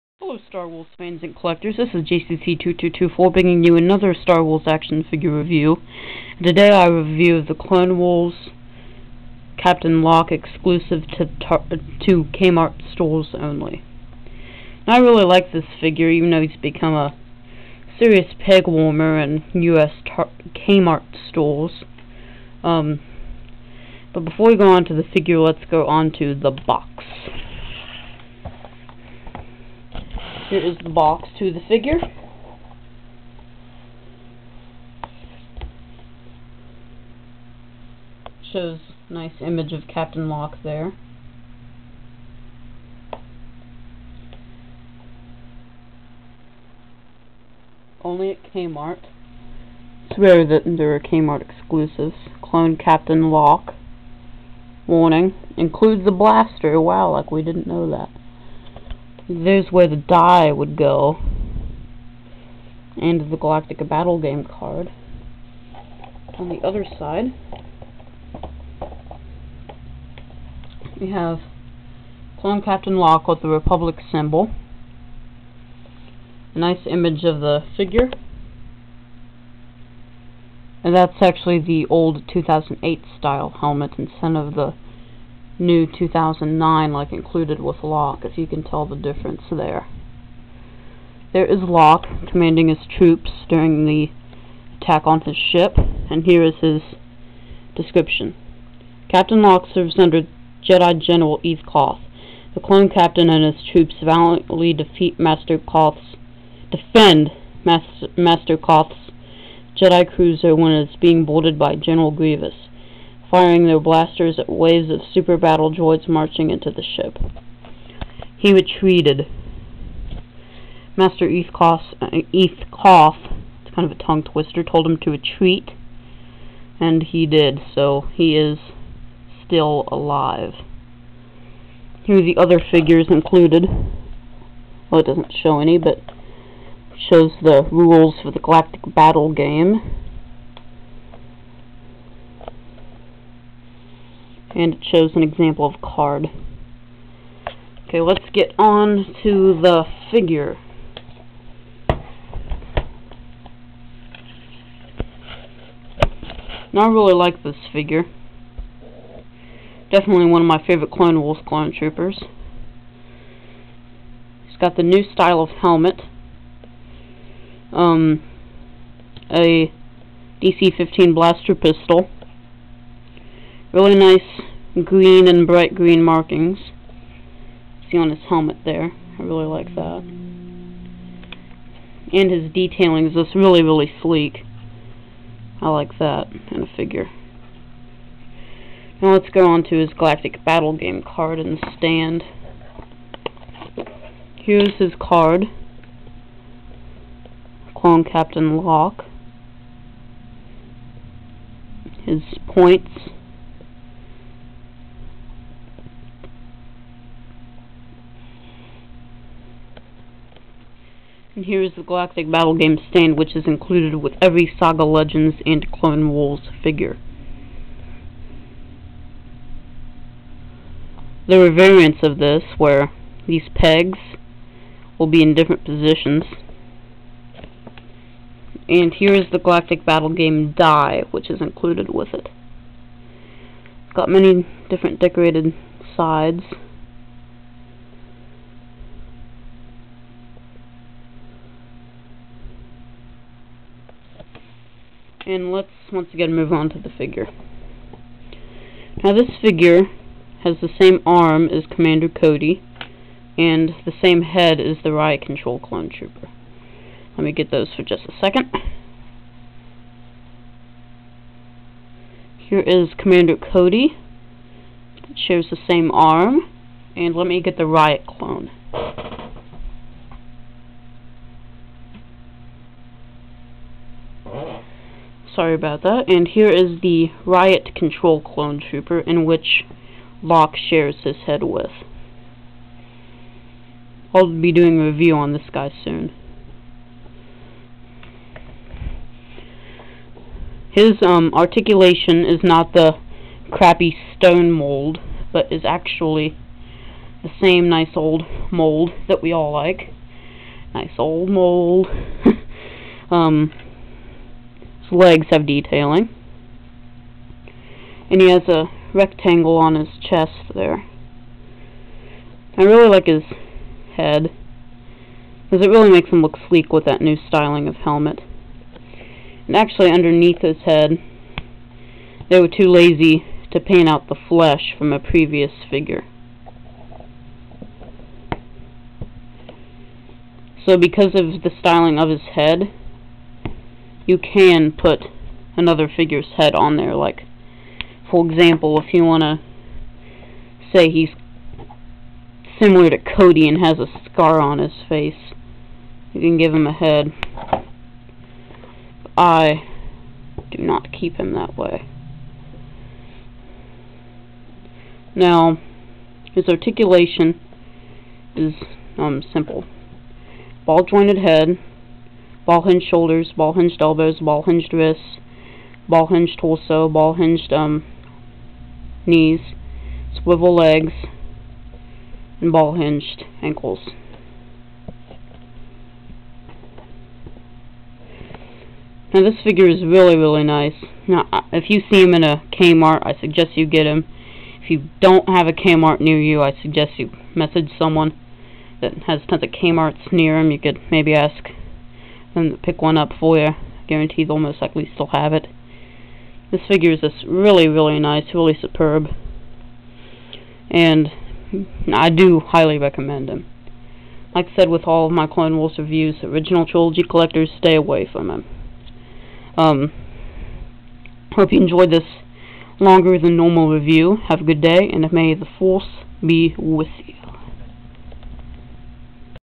Hasbro Star Wars The Clone Wars: K-mart Exclusive Captain Lock
Captain Lock serves under the Jedi General Eeth Koth. The Clone Captain and his troops valiantly defend Master Koth's Jedi Cruiser when it is being boarded by General Grievous, fuiring their blasters at waves of super battle droids marching into the ship.

This is my first review of a CW exclusive

I don't want to mislead anyone, due to time and shipping costs, I will not send out the Codes, but if you guys want the code numbers, I will most likely send them to you in a message.